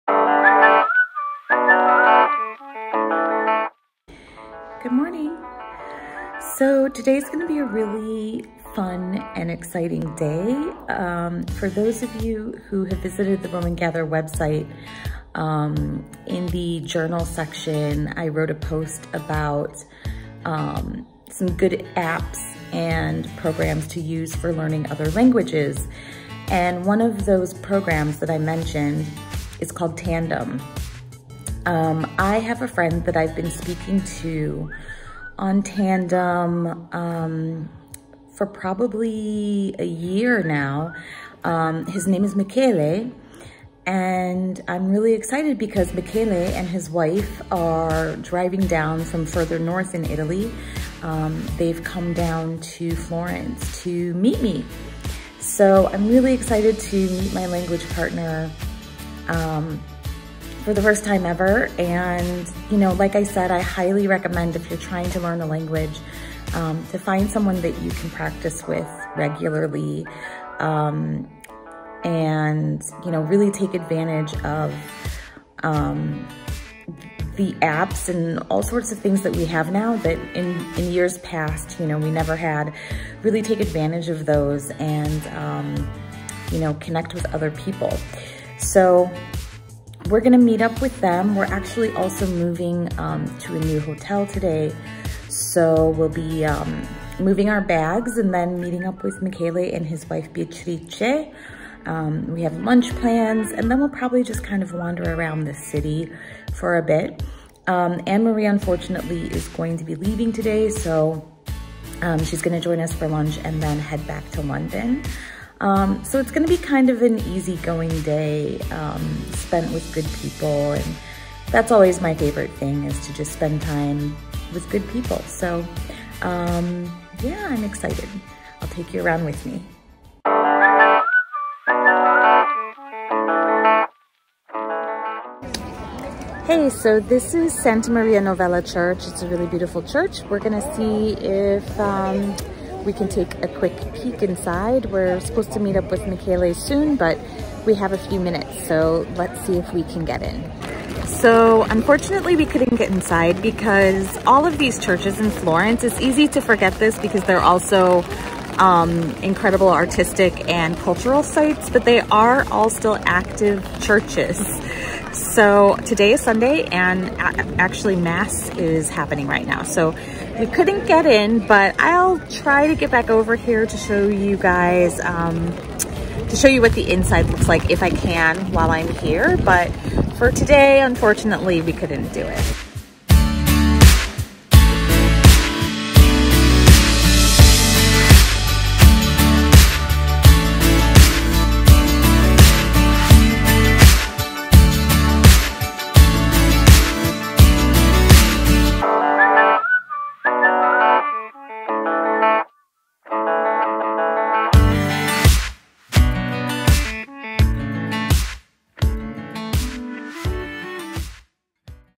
Italy Vlog | Day 6
Today we had terrible weather in Florence. There was a lot of rain and wind, and I was afraid to pull out my video equipment and get it soaked.

So it's a short one today. My friend Michele and his wife drove down from a town called Alessandria to meet me. I met Michele through a language exchange app called Tandem, and we've become great friends.

I highly recommend using a language exchange app if you're trying to improve or learn a language. You never know what great friendships will develop!